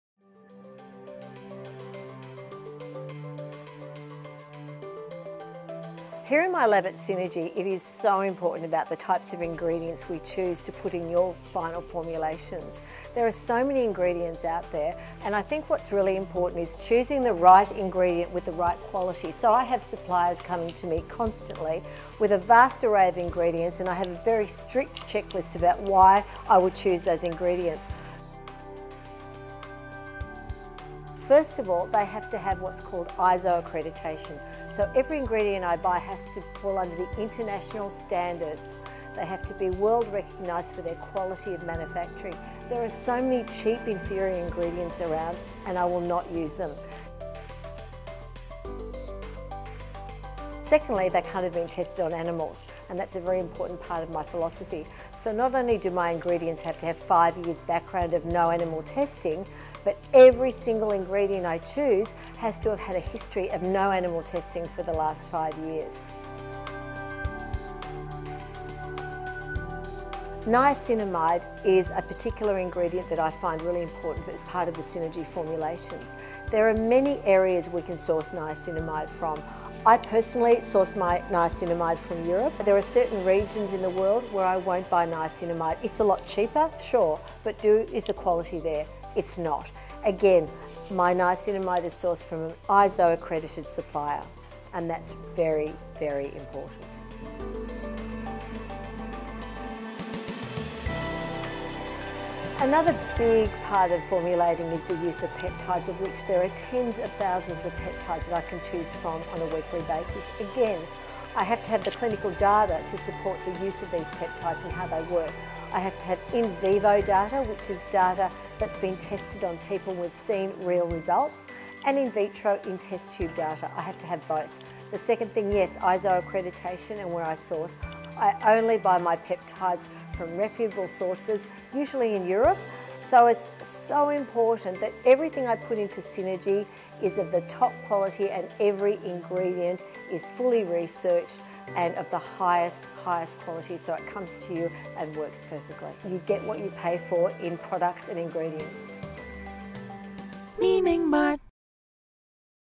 走進 Synergie 實驗室｜Terri 公開成分的秘密
嚴選成分是Synergie Skin® 創辦人和配方研製師 Terri Vinson一直秉持的首要原則，其成分必須符合以下4大標準😠

1）ISO國際品質標準🎓
Syenrgie產品的每一種成分均符合國際品質標準，製造質量是為世界所公認的。

2）5年以上無動物測試背景🐰
Synergie的產品不作動物測試外，其成分亦必須擁有5年以上無動物測試的背景，堅持美麗零傷害。

3）嚴選優質產地🌎
拒絕使用廉價品質差的成分供應商，Synergie只選用優質成分，保障產品的效用性和您的安全。Synergie Skin®配方中的維他命B3 (niacinamide)來自歐洲👐🏻

4）Vivo & Vitro 臨床數據實證🔬
肽 (Peptides)在Synergie的配方中擔當重要的角色，為確保科研成分的成效及安全，它們均經過人體皮膚測試 (Vivo Test) 及科學試管測試 (Vitro Test)。